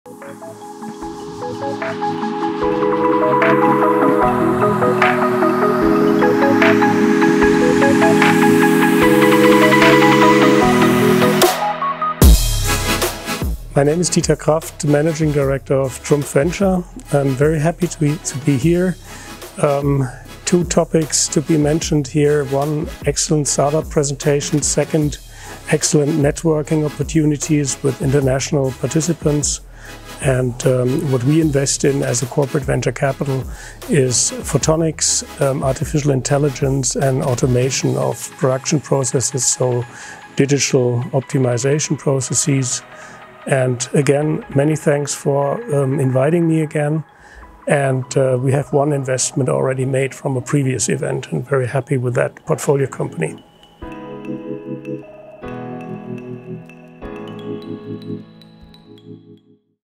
TOP 100 Investor Summit 2023 - Dieter Kraft, Managing Director Trumpf Ventures
Switzerland is home to excellent startups and according to the Global-Innovation-Index the most innovative country for many years in a row. There is a unique opportunity to discover the most promising Swiss startups! An exclusive invitation-only TOP 100 Investor Summit was followed by the TOP 100 Swiss Startup Award on September 6, 2023, to meet handpicked Swiss startups raising a financing round.

Dieter Kraft, Managing Director Trumpf Ventures shares his impressions with us.
"My name is Dieter Kraft, managing director of Trumpf Ventures. I'm very happy to be here. Two topics to be mentioned here. One: excellent startup presentation. Second: excellent networking opportunities with international participants. What we invest in as a corporate venture capital is photonics, artificial intelligence and automation of production processes. So digital optimization processes. And again, many thanks for inviting me again.
And we have one investment already made from a previous event and very happy with that portfolio company."